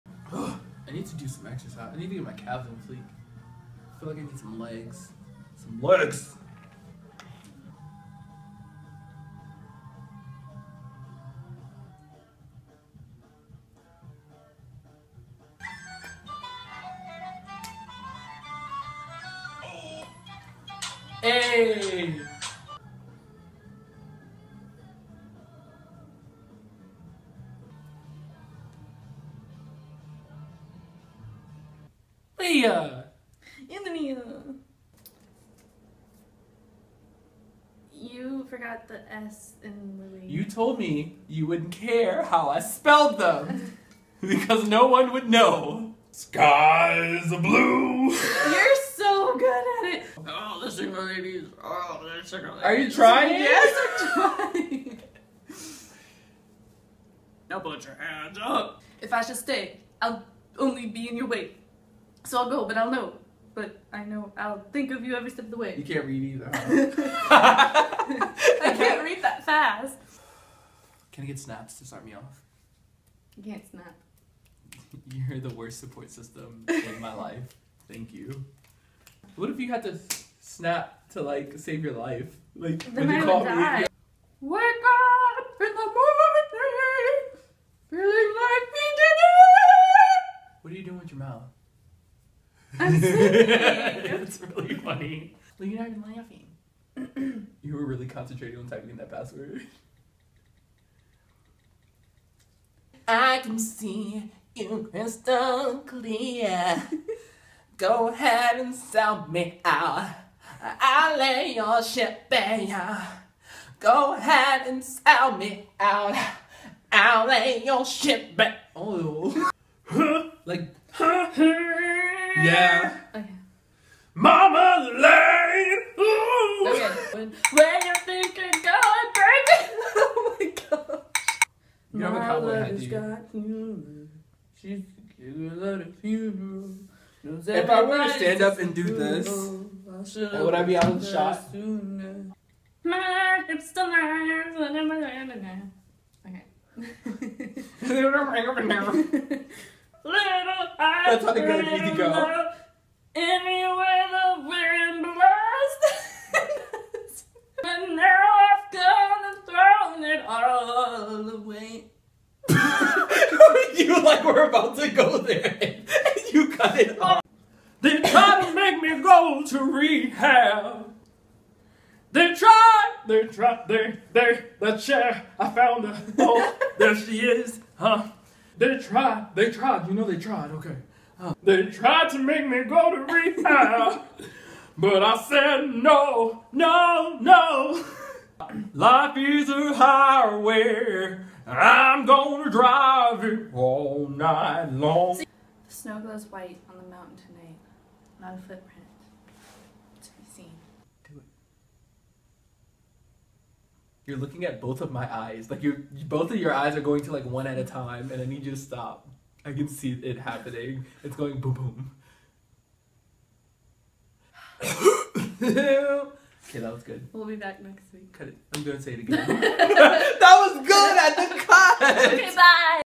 Spoiler Bloopers
This is a sneak peek for the next video which will be going up soon! We've been having technical difficulties.